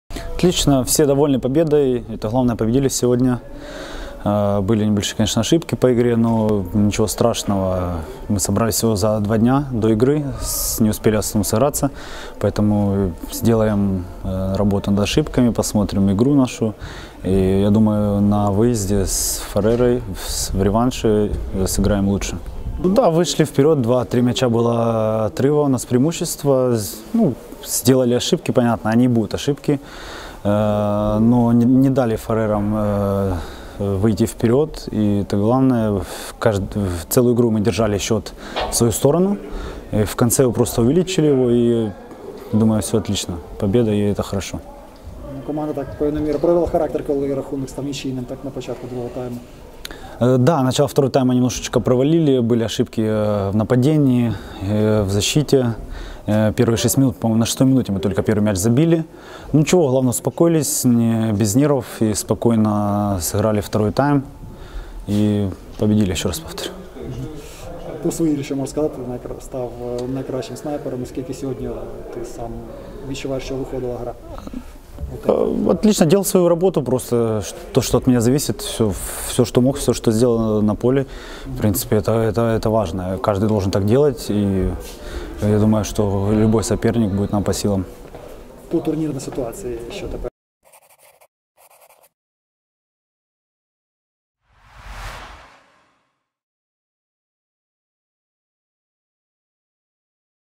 Андрій Акіменко: "У нас були помилки, але ми не дали Фарерами вийти вперед"
Коментар Андрія Акіменка (7 голів) після перемоги збірної України над Фарерськими островами (25:21) у відборі на Євро-2021